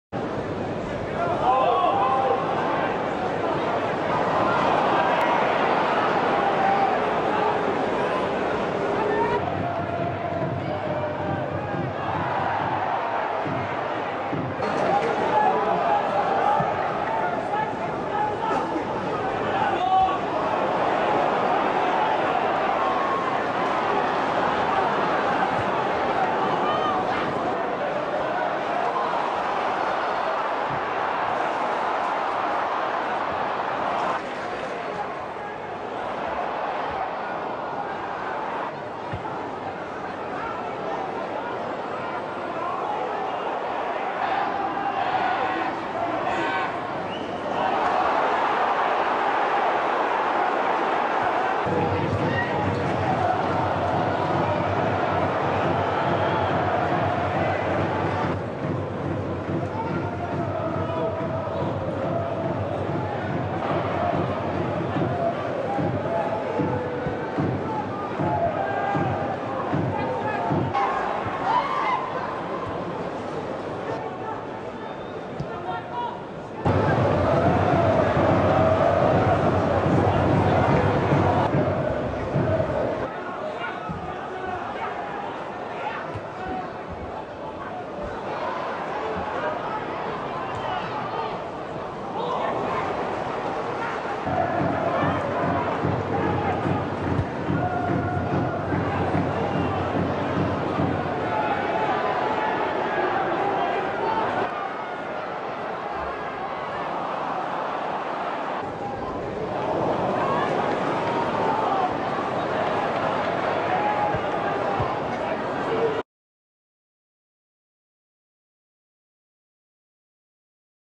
Genseric KUSUNGA vs Hibernian (24/11/2018)
Highlight of Genseric Kusunga against Hibernian, Scottish Professional Football League (SPFL).